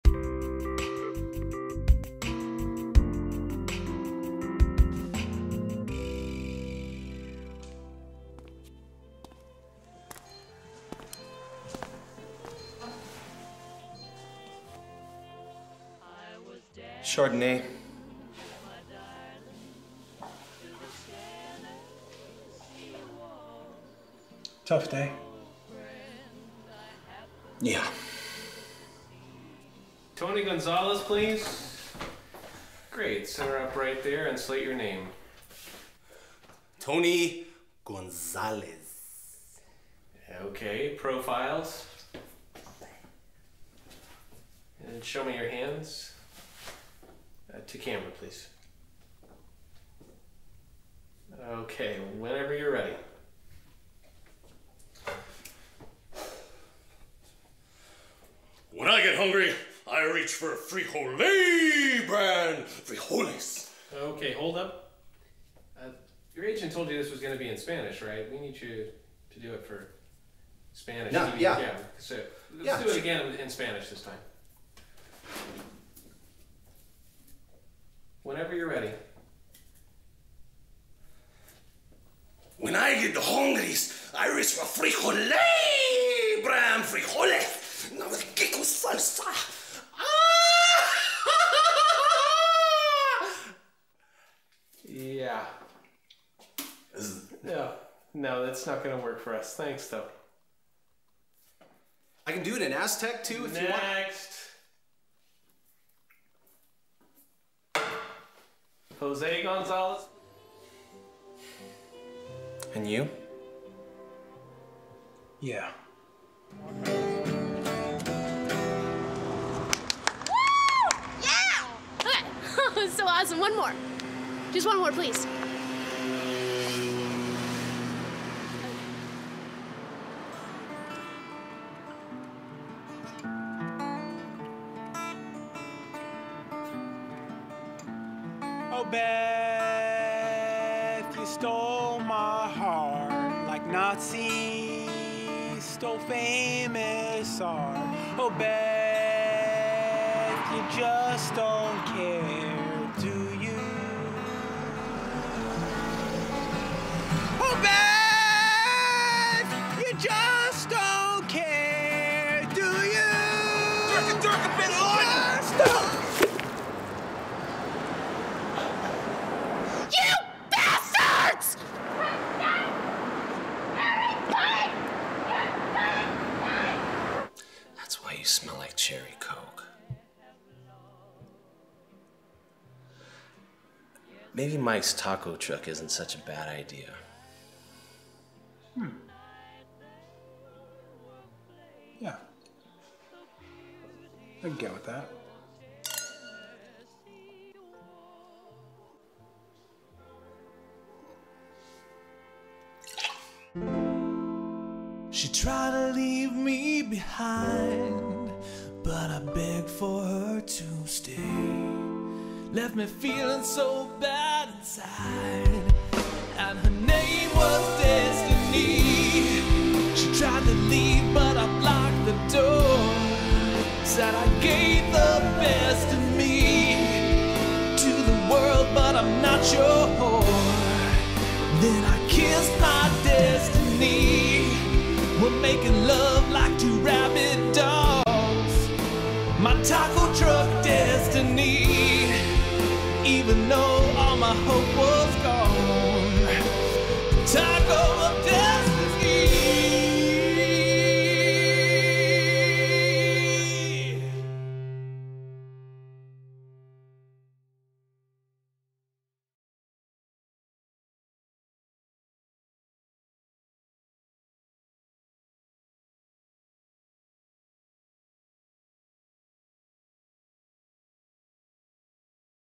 You Love I. episode 2
Mose and Tony rethink Mike's Taco Truck idea.